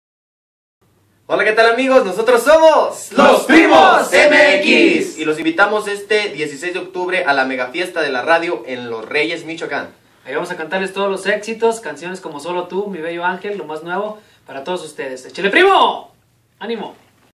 Saludos Los Primos MX Mega Fiesta de La RAdio
Aqui les dejamos la invitación de parte de Los Primos MX a la Mega Fiesta de la Radio este proximo Miércoles 16 de Octubre en los Reyes Michoacán, ademas estara Banda La Autentica, Elias Medina, Banda Maizera, Charanda Band, Musical Hilarios y su Onda Chicana, Los Idolos de Mexico y Ezequiel y su tierra firme, Stereo Mia 92.5 FM y 1000 AM te invita!!